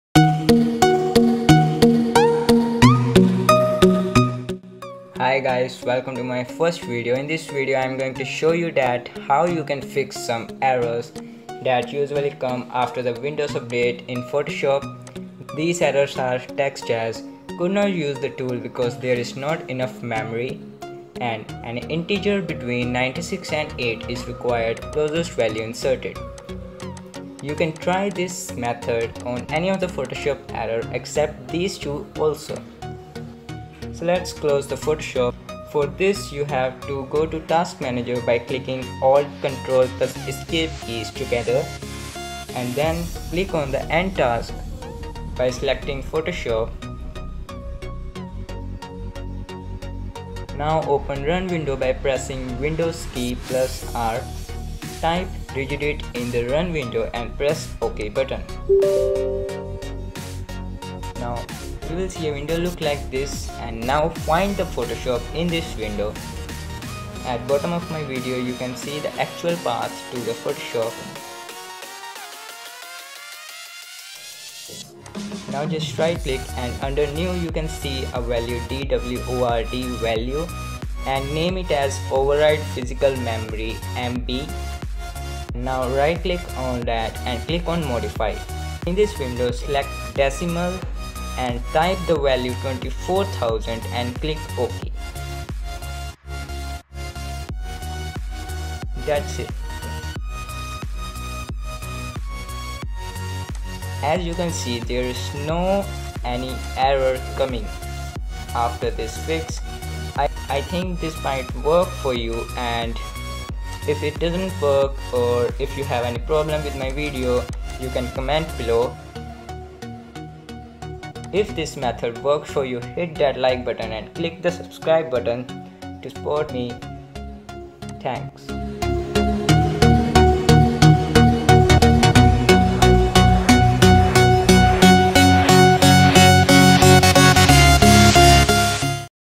Fix Photoshop CC Error | Not enough memory RAM
This video will show you how to fix the following Photoshop CC errors:
1) Could not complete the Image Size dommand because there is not enough memory (RAM).
2) An integer between 96 and 8 is required. Closest value inserted.

The problem can be fixed by creating a registry d-word 32 bit value "OverridePhysicalMemoryMB" with "decimal base" with a value equivalent to your ram size in megabyte.  (I had made mistake in this video and choose "hexadecimal base" instead of decimal but hexadecimal will also work). For example if you got 2gb ram then you can put the value 2x1024mb=2048 (since 1gb=1024mb). Likewise 4gb=4x1024=4096, 8gb=8x1024=8192, 16gb=16x1024=16384 and so on.

If you have problem in choosing correct registry key. The following will help you choose the correct key:
(i)  Photoshop CC 2014       :    80.0
(ii) Photoshop CC 2015       :    90.0
(iii) Photoshop CC 2015.5   :   100.0
(iv) Photoshop CC 2017      :    110.0
(v) Photoshop CC 2018       :    120.0